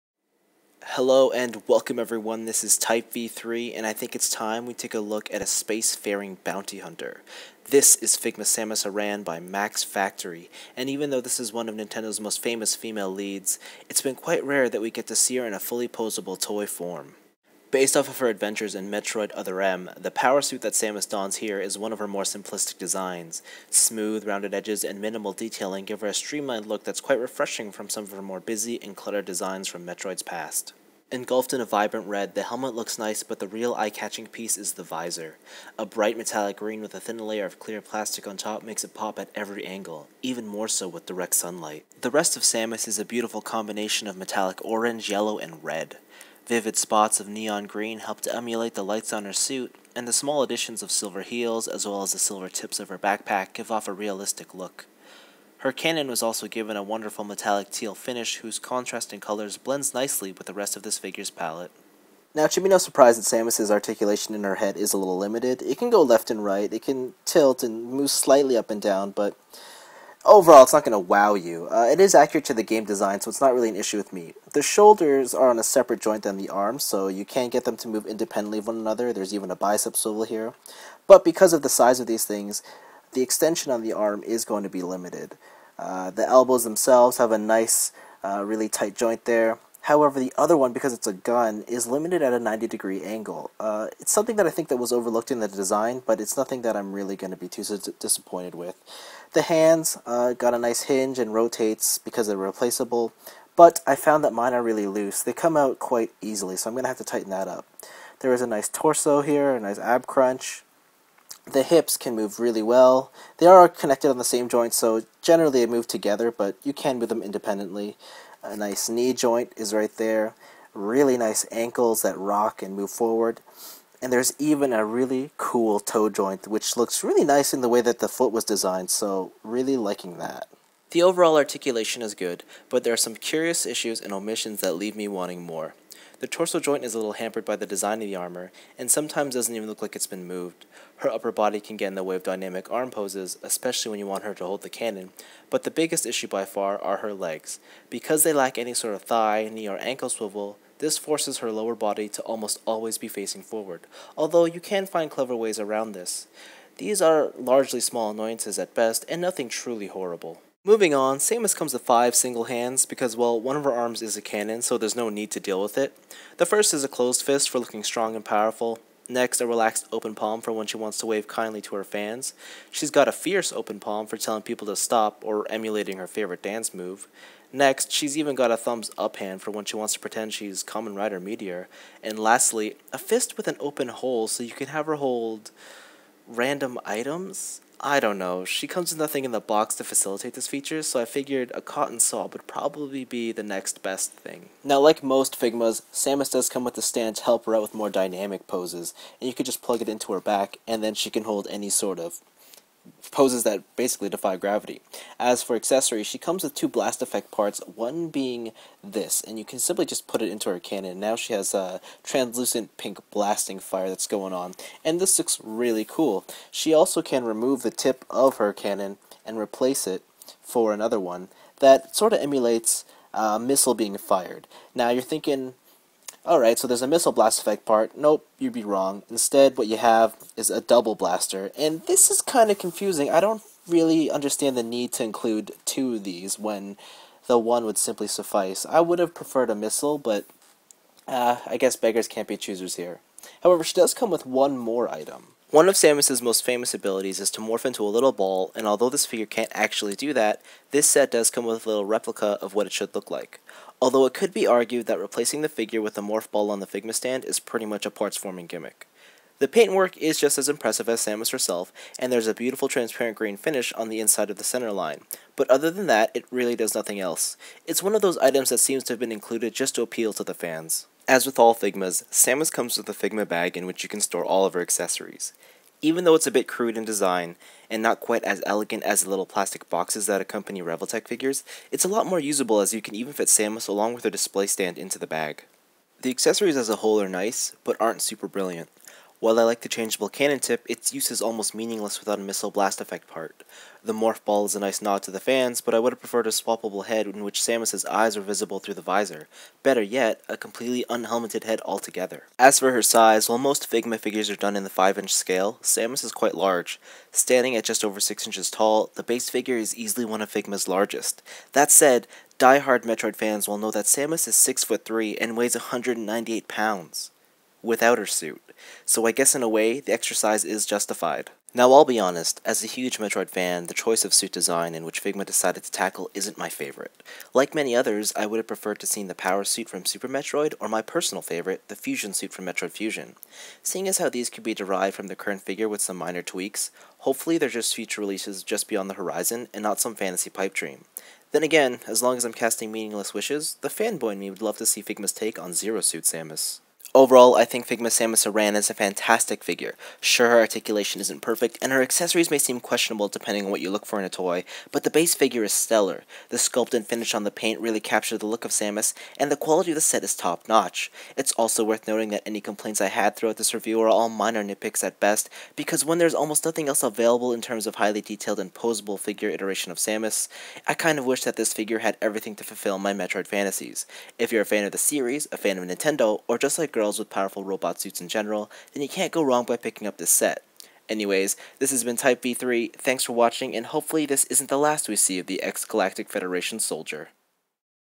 Figma Samus Aran (Metroid Other M) | REVIEW
Does Figma's take on Nintendo's famous female bounty hunter live up to the legacy that precedes her?